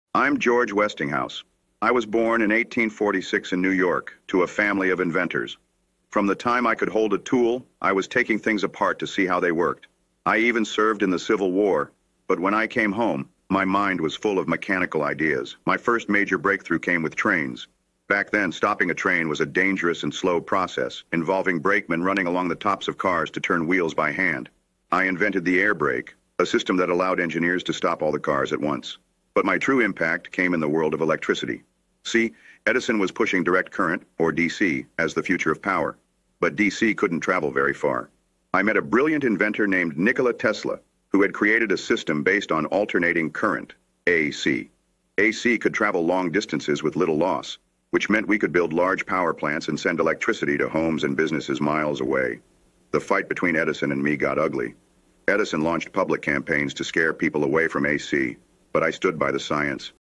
George Westinghouse Tells of His Battle with Edison Over DC and AC Current, Telsa's Energy
My name is George Westinghouse, and if you’ve ever turned on a light, hopped on a train, or walked through a modern city powered by electricity, then you've lived in the world I helped build. I wasn’t a flashy man, and I didn’t always seek the spotlight, but I believed in ideas—especially the ones that could make life better, safer, and more efficient for everyone. Let me tell you how I became known as “The Industrial Strategist.”

I was born in 1846 in Central Bridge, New York, to a family of inventors and craftsmen. From the time I could hold a tool, I was taking things apart to see how they worked—and usually putting them back together a little better. I served briefly in the Civil War, but when I came home, my mind was full of mechanical ideas. I filed my first patent at just 19 years old for a rotary steam engine. But my first major breakthrough came with trains. Back then, stopping a train was a dangerous and slow process, involving brakemen running along the tops of cars to turn wheels by hand. I invented the air brake—a system that allowed engineers to stop all the cars at once from the locomotive. It saved lives and revolutionized the railroad industry.

But my true impact came in the world of electricity. You see, during the 1880s, everyone was talking about Thomas Edison and his new electric lights. Edison was pushing Direct Current—or DC—as the future of power. But DC couldn’t travel very far. You had to build a power plant every mile or so. That’s where I came in. I met a brilliant inventor named Nikola Tesla, who had created a system based on Alternating Current—AC. AC could travel long distances with little loss, which meant we could build large power plants and send electricity to homes and businesses many miles away. I saw the potential and bought the rights to Tesla’s patents.

Now, I won’t lie to you—the fight between Edison and me over AC vs. DC got ugly. Edison launched public campaigns to scare people away from AC. He even supported the use of AC in the first electric chair just to show how dangerous it was. But I stood by the science. When we lit up the 1893 World’s Fair in Chicago using AC, the world saw the truth. Not long after, we built a power plant at Niagara Falls that brought electricity to Buffalo, New York. That was when AC officially won.

But I didn’t stop there. I started over 60 companies in my lifetime—working on everything from electric railways to natural gas systems. I believed in treating my workers fairly and always aimed to use technology to improve lives. I wasn’t just focused on machines. I cared deeply about the people who built and benefited from them.

I passed away in 1914, just before the world plunged into war. But I had already seen the impact of what we’d built: a connected, electrified, and rapidly changing world. People today remember Edison’s name, and Tesla’s fame has grown over the years, but I like to think of myself as the one who brought the pieces together. I took great ideas and turned them into real systems that worked for everyone.

So if you ever feel like the quiet kid with the practical ideas, remember this—sometimes the one who gets things done changes the world more than the one who grabs the headlines. That was me. That was George Westinghouse.